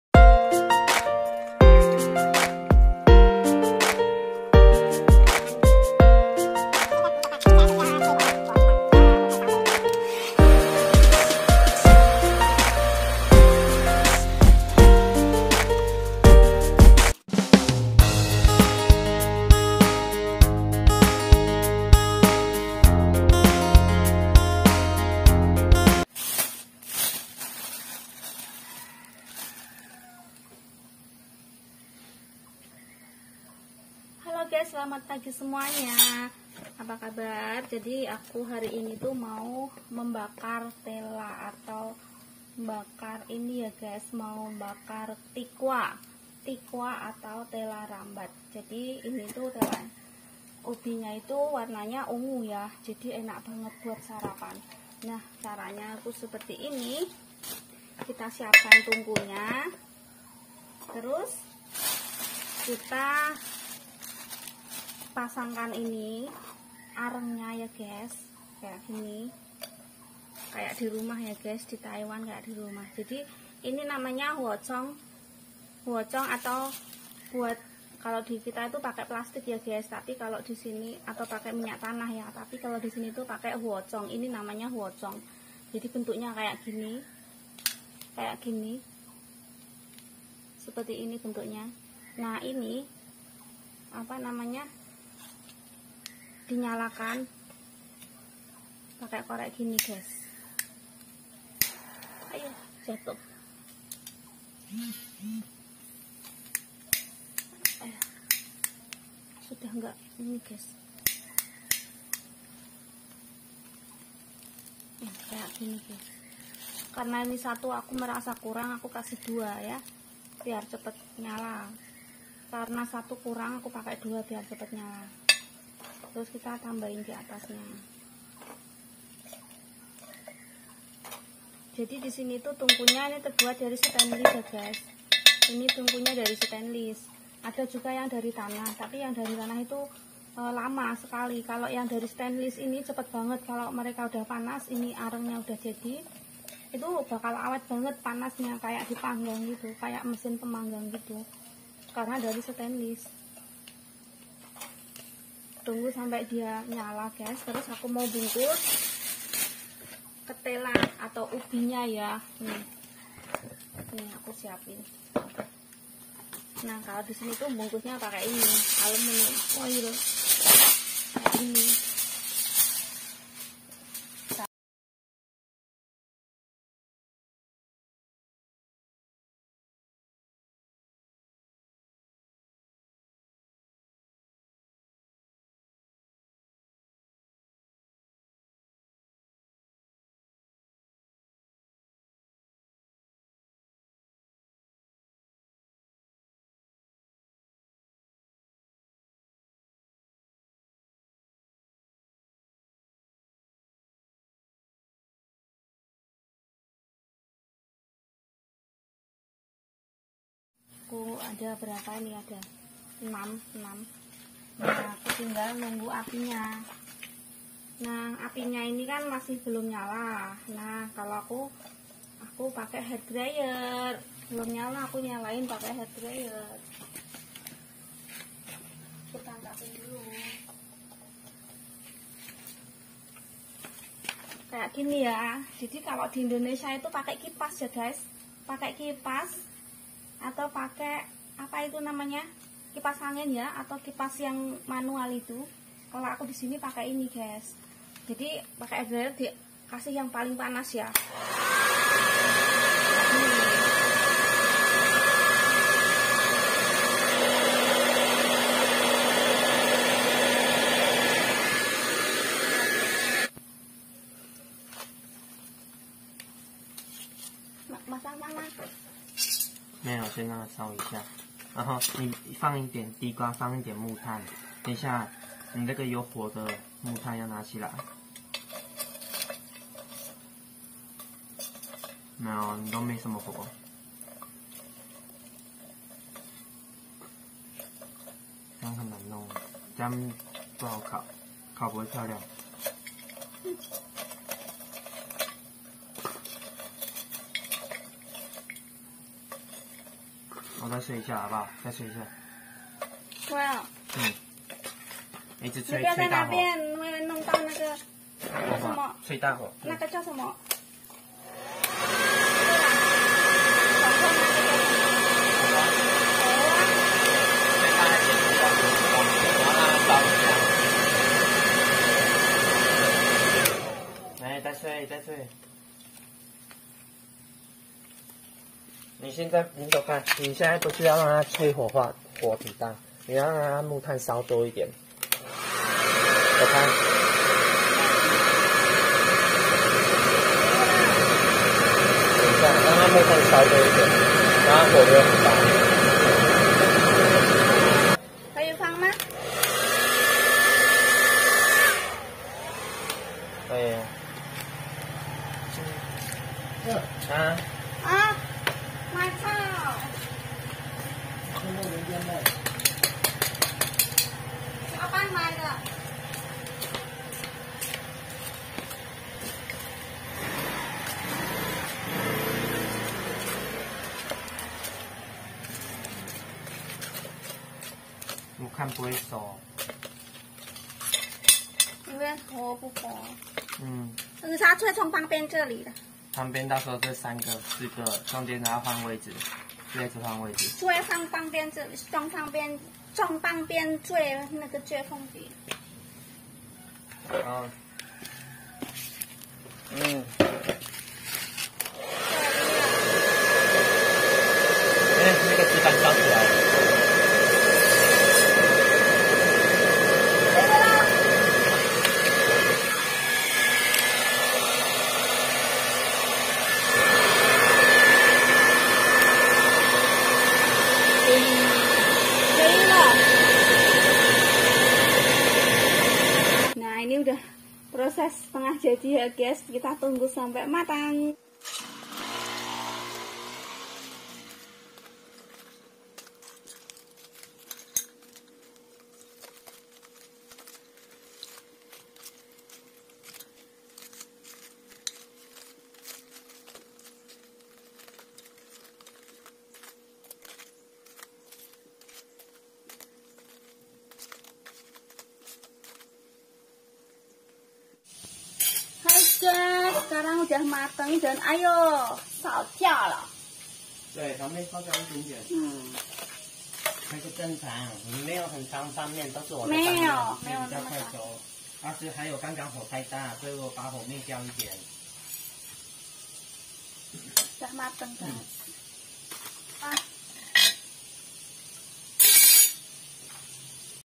Tutorial Mbakar TELO di Taiwan
hai jumpa lagi di vidio q kali ini aq sedang bakar telo atau ubi
yuk tonton bgmn cara aq bakar
di bantuin TITI
praktis di tiup pakai hairdryer

terima kasih yg udah nonton
Bakar Telo
TKW TAIWAN